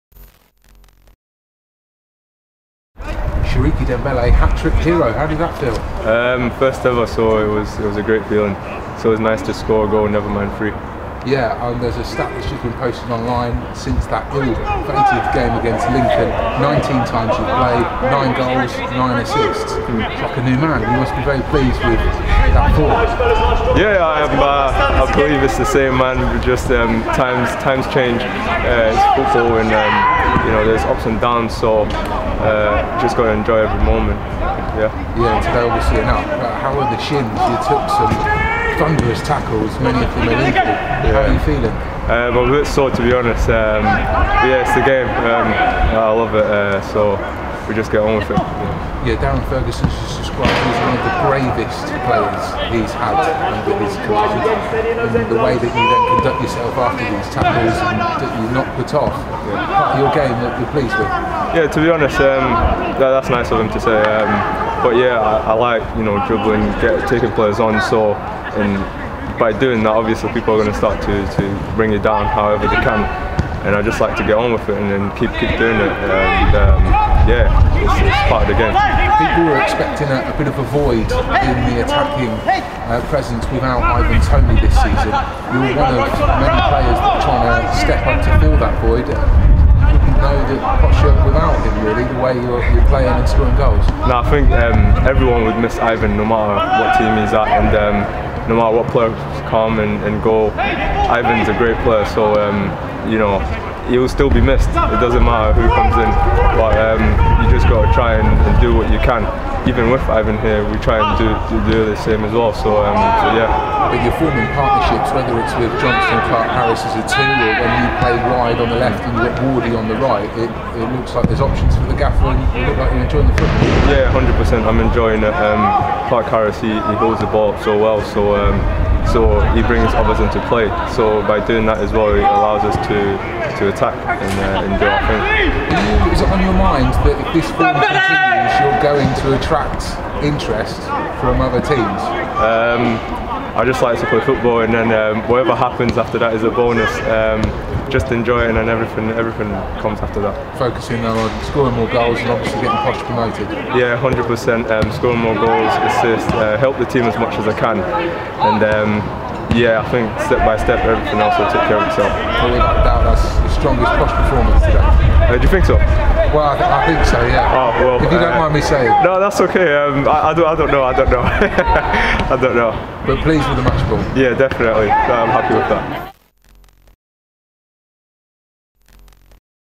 Dembele Delighted With First Professional Hat-trick
Hat-trick hero Siriki Dembele spoke to the media following the 5-1 win over Shrewsbury Town at the Weston Homes Stadium.

Twitter | Facebook | Instagram - @ThePoshOfficial